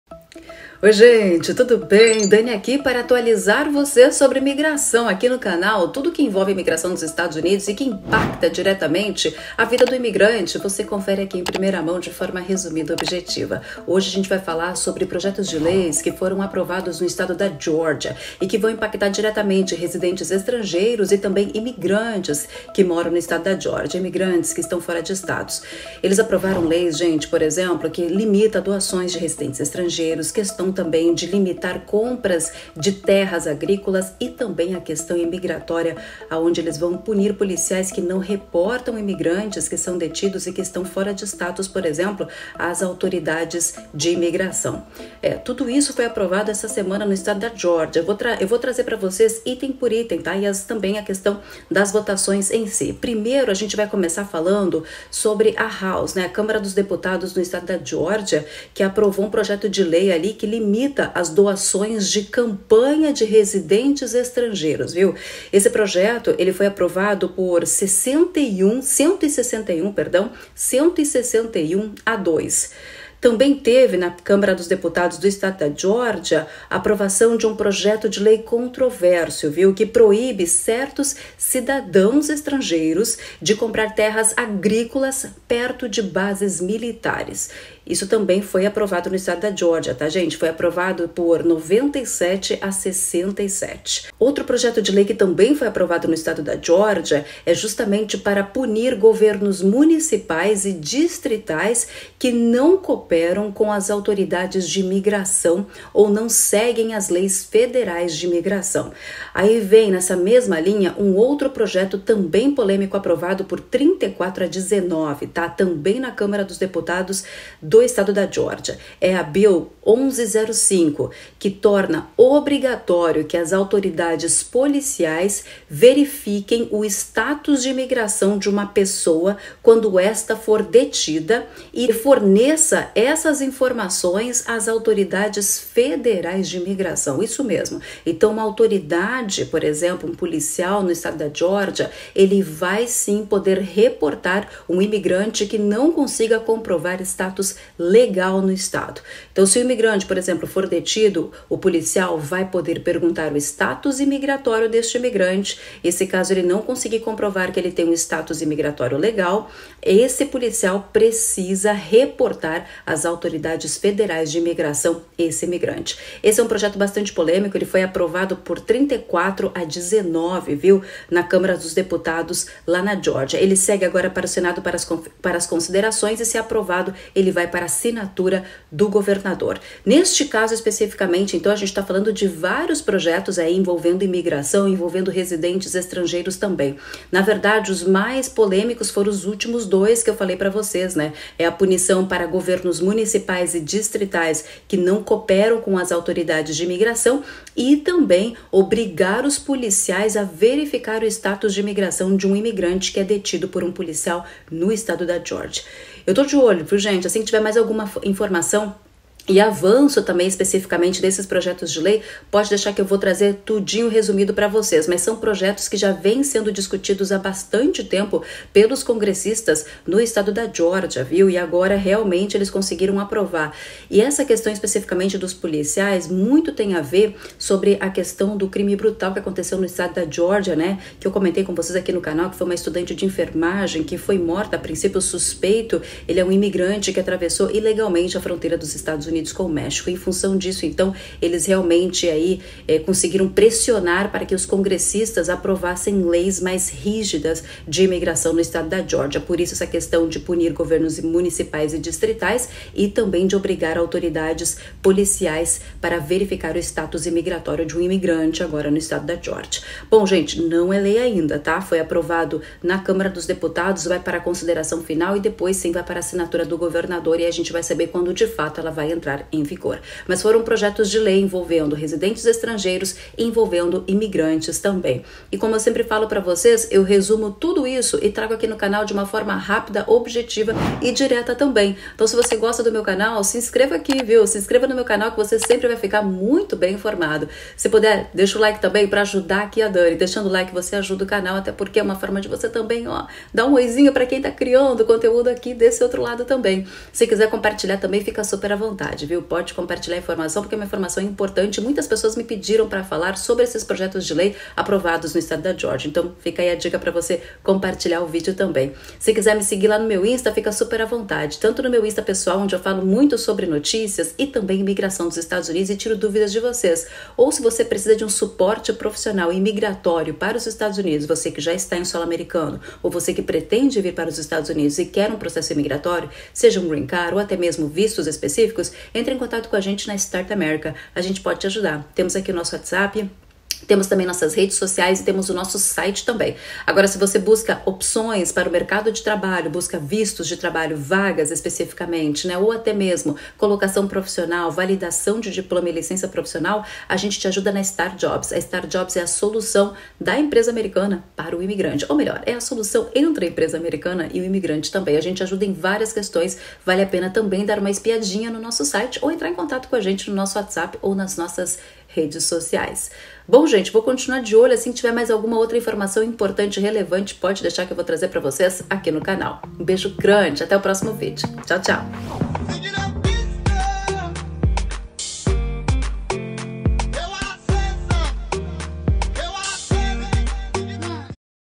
MAIS UM ESTADO APROVA LEI CONTRA IMIGRANTES ILEGAIS NOS EUA!
A POLÍCIA VAI SER OBRIGADA A VERIFICAR O STATUS DO IMIGRANTE!

📝 Quer meu suporte para ESTUDO, TRABALHO, CARREIRA E VISTO nos Estados Unidos?

👾 Meu aplicativo gratuito: USA by Dani Azeredo